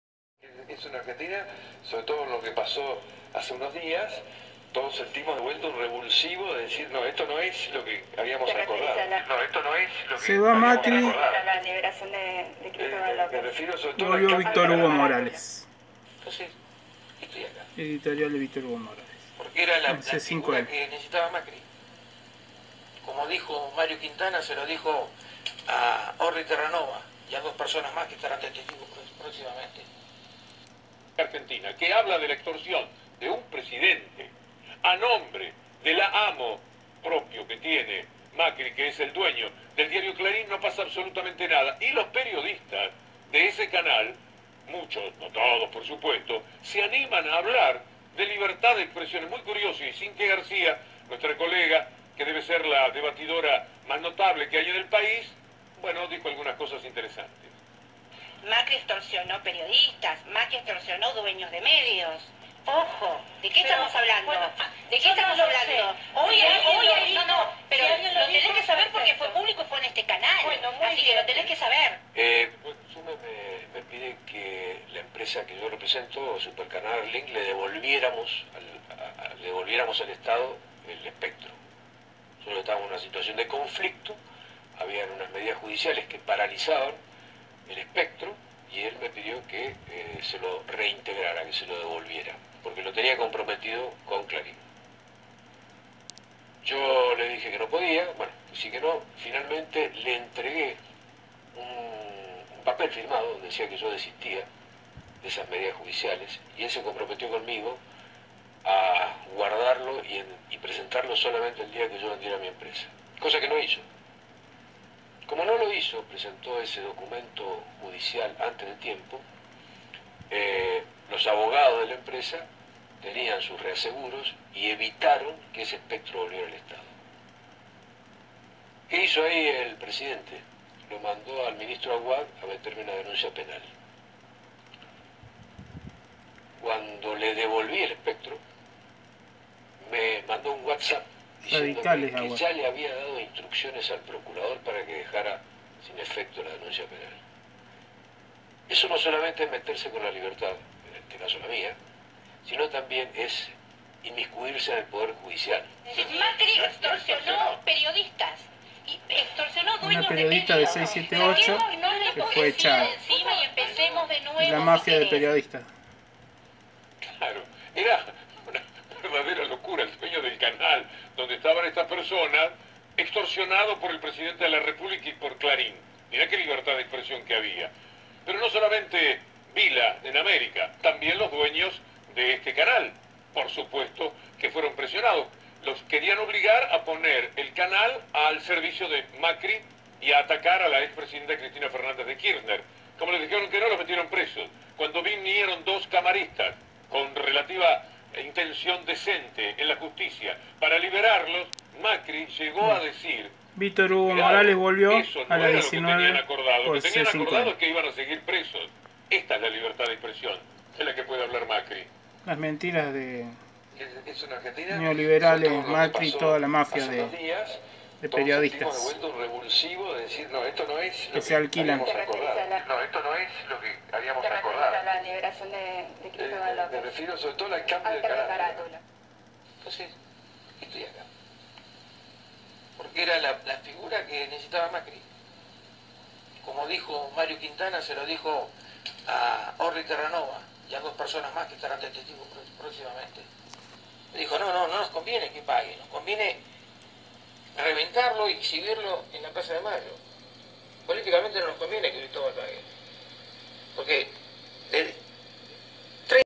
Volvio VHM👏 a C5N 📹📺Se va un mentiroso📻😾💩
Del Canal de Youtube C5N del 09/12/2019 Victor Hugo Morales: Editorial